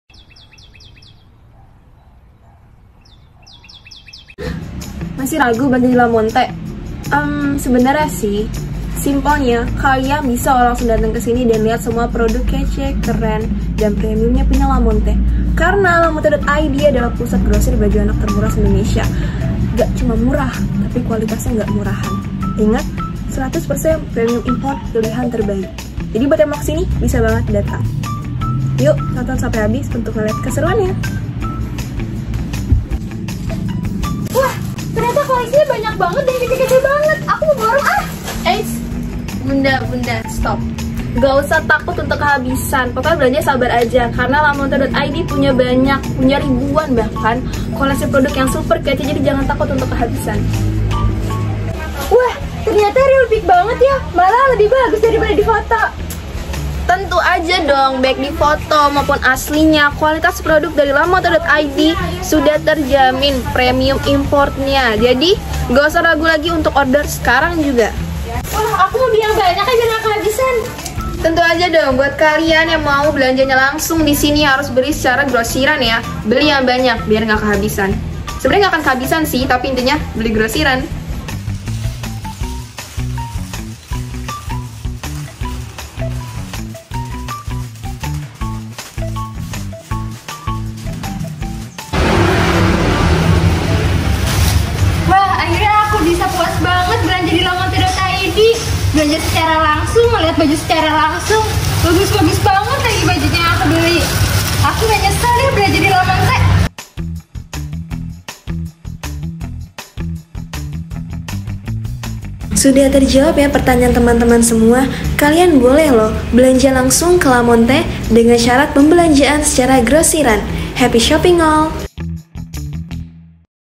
MEMANG BOLEH BELANJA LANGSUNG KE LAMONTE?
Belanja langsung ke Lamonte?
Memang bisa?
Semua pertanyaan kamu akan dijawab di video ini, ya!

Lamonte.id